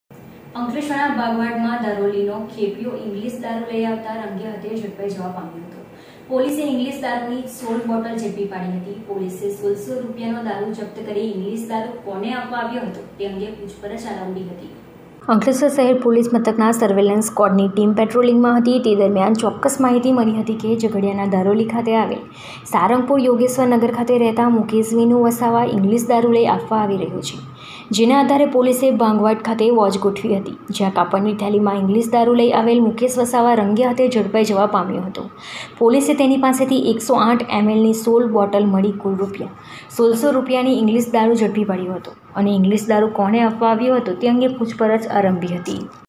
માઁ ટીવી 02.05.2022 દારૂ ઝડપાયો : અંકલેશ્વરના ભાગવાડ માં ધારોલી નો ખેપિયો ઝડપાયો
દારૂ ઝડપાયો : અંકલેશ્વરના ભાગવાડ માં ધારોલી નો ખેપિયો ઝડપાયો
પોલીસ એ ઈંગ્લીશ દારૂની 16 બોટલ ઝડપી પાડી : પોલીસે 1600 રૂપિયા નો દારૂ જપ્ત કરી ઈંગ્લીશ દારૂ કોને આપવા આવ્યો હતો તે અંગે પૂછપરછ આરંભી હતી